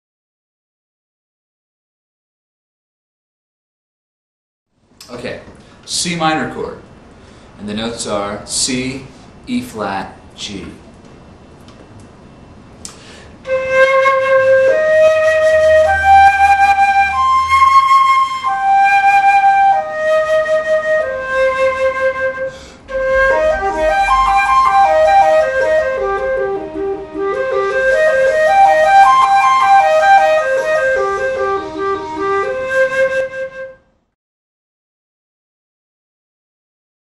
Flute Lesson: How to play a C Minor Chord with Arpegio
Beginner lesson: This is a short lesson on how to play a C minor chord on the flute with arpegio, Dan Lefler of
Danman's Music School demonstrates how it should sound when played correctly. For 1000's more flute lessons and the lesson notes, got to
Danmans.com (file: 200)